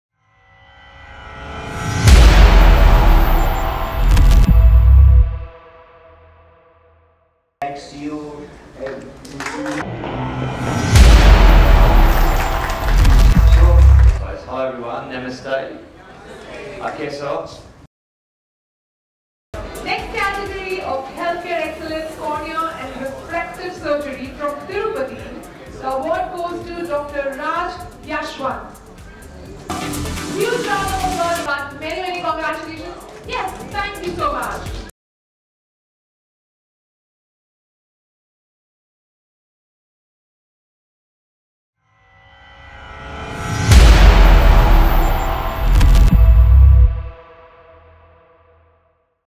Dr. Raj Yashwant at the India Brand Icon Awards, 2024 | Mrs. Brett Lee
gets recognised at the India Brand Icon Awards, 2024 by Time Cyber Media Pvt Ltd in Mumbai in Mumbai in presence of Former Australian Cricketer Mr. Brett Lee

on stage to received the award.